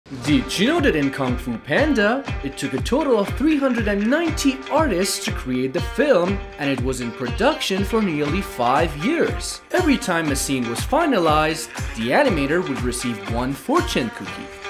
Did You Know That In Kung Fu Panda
It took a total of 390 artists to create the film And it was in production for nearly 5 years. Every time a scene was finalized, The animator would receive one fortune cookie.